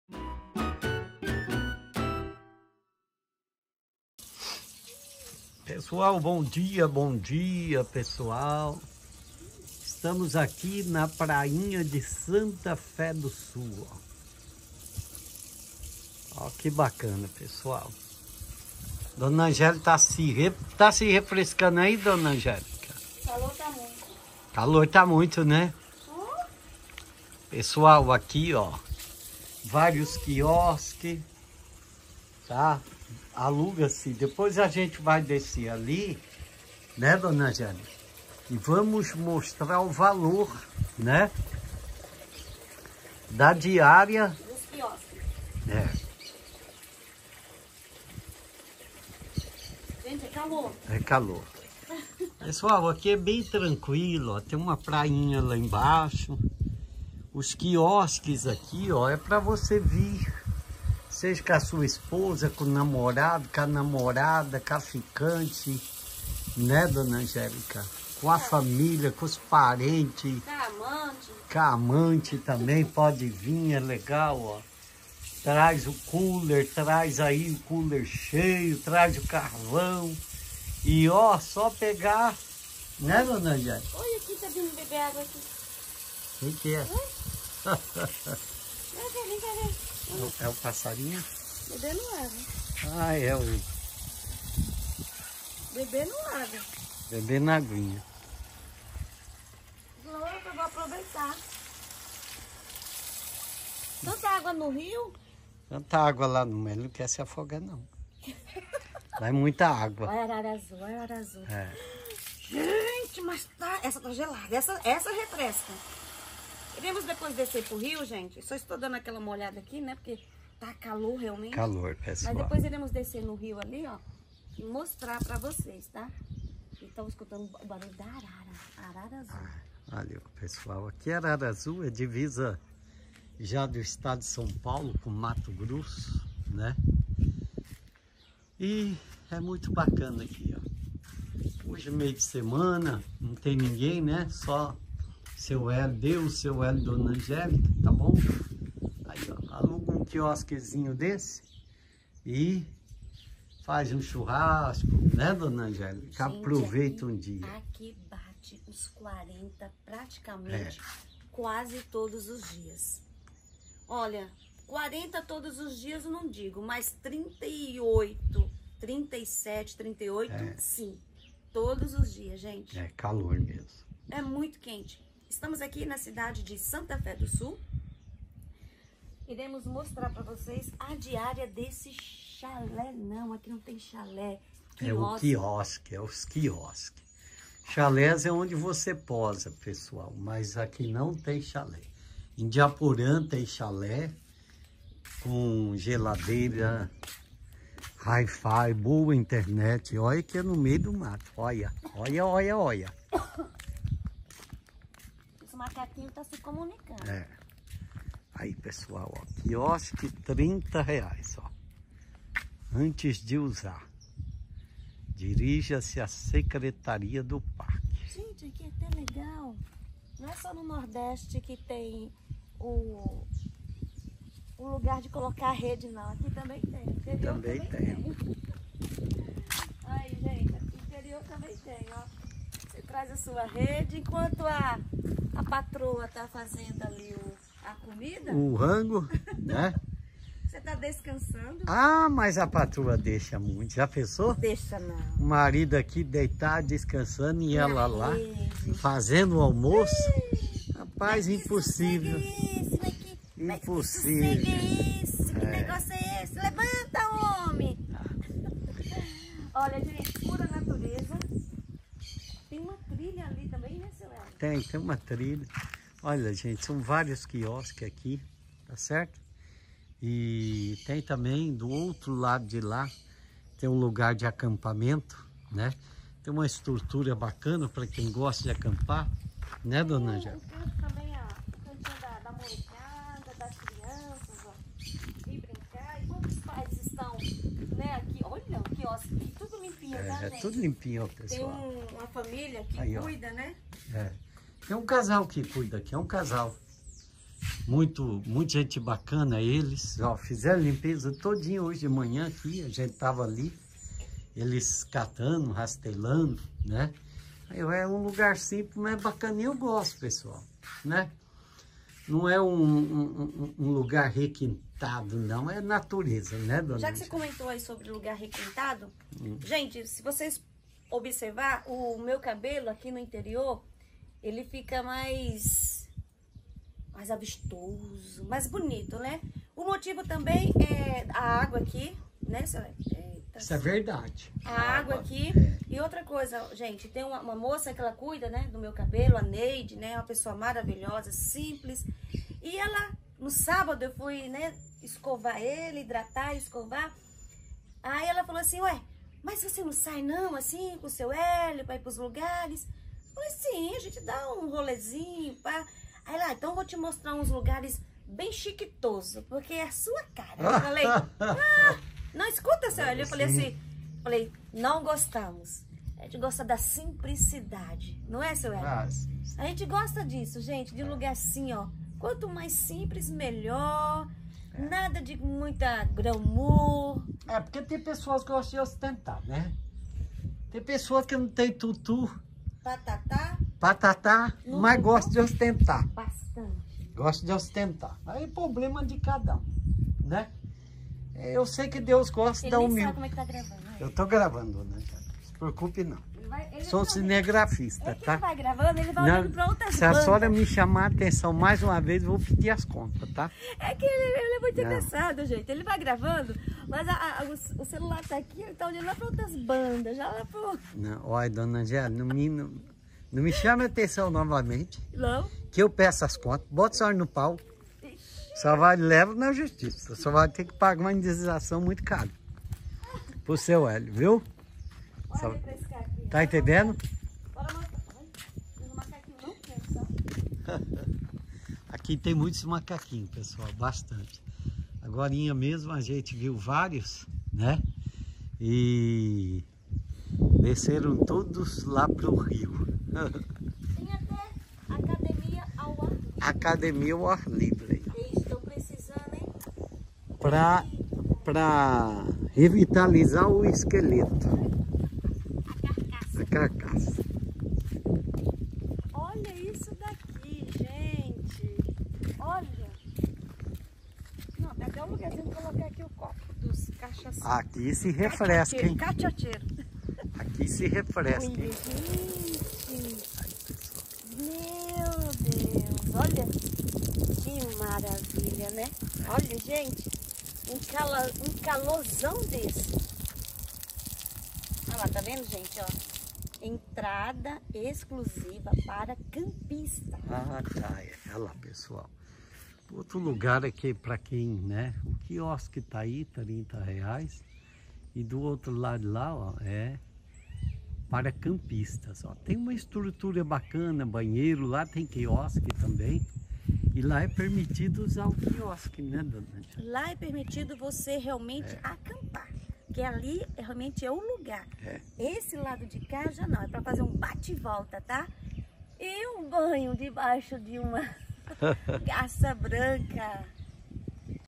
Parque ecoturístico águas claras cidade de Santa Fé do Sul
Parque Ecoturistico aguas claras cidade de Santa fé do sul no rio Paraná muito bom esta lugar agradeço e fiquem com Deus!

Beijos meu amores, fiquem com Deus!!